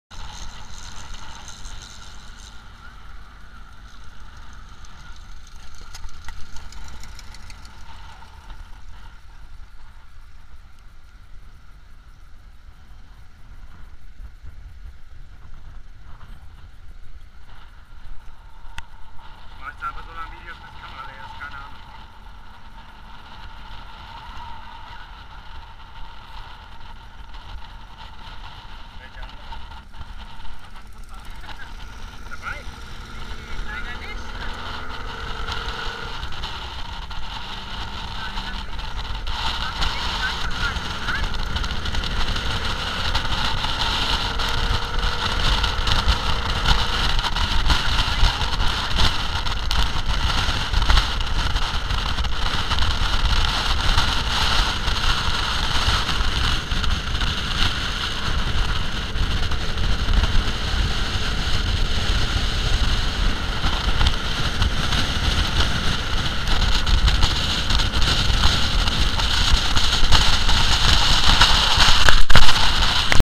Fahrt mit einem VW Buggy am Strand in Jericoacoara
Fahrt mit einem VW Buggy am Strand in Jericoacoara in Brasilien.